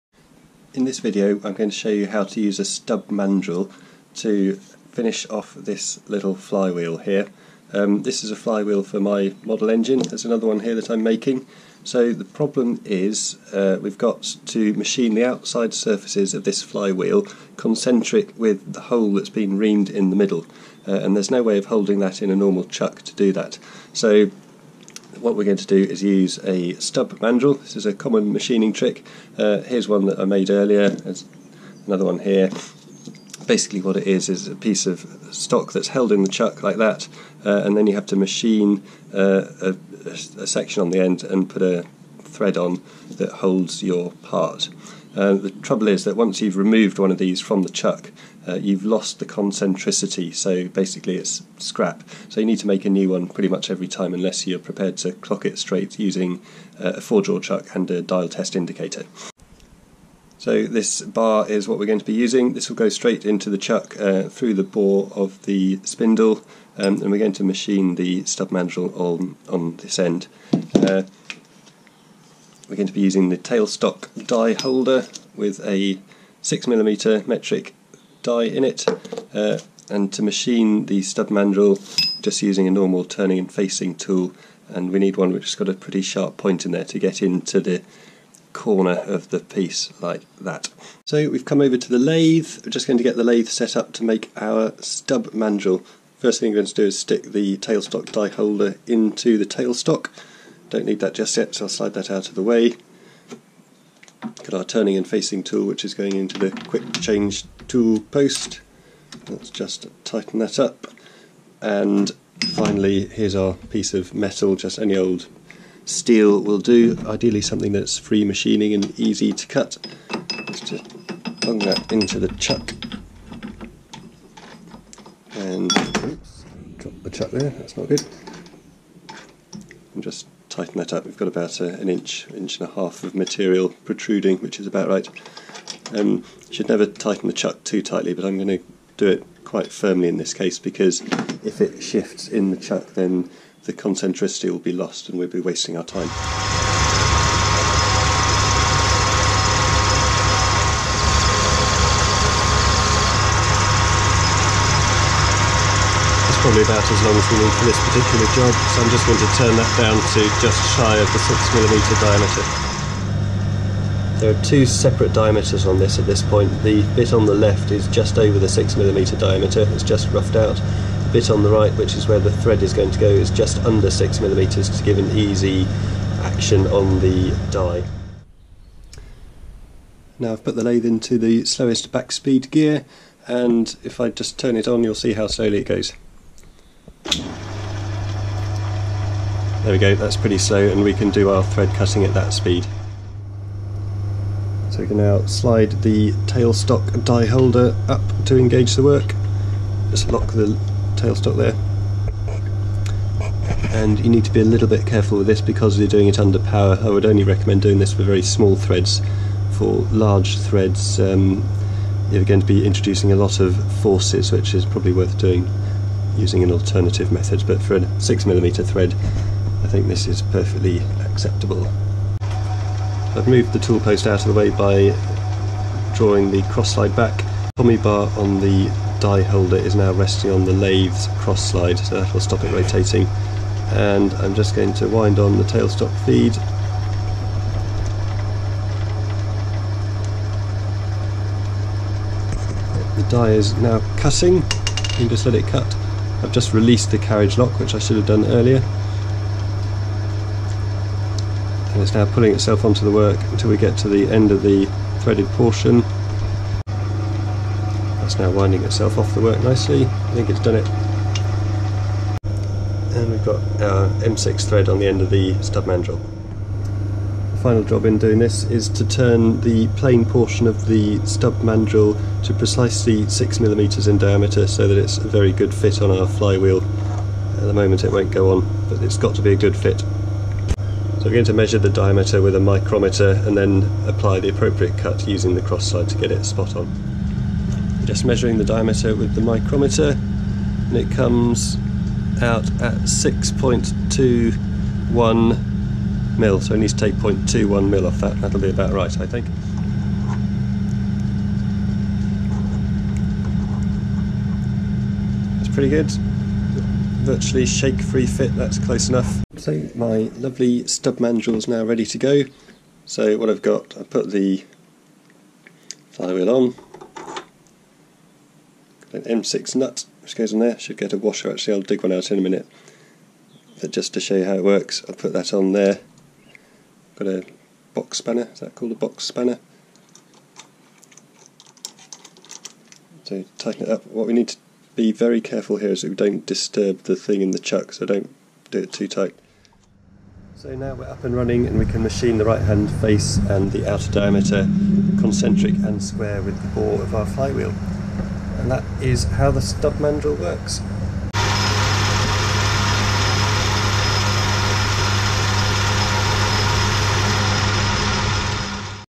Making a Stub Mandrel on the Myford Lathe
A stub mandrel is used on the lathe to hold a part by its bore, allowing all the external surfaces to be machined.  In this video I explain what a stub mandrel is, how to make one and how to use one.
If you are a machinist, you need stub mandrels in your life!  They provide a quick, easy and accurate workholding method for the metal turning lathe.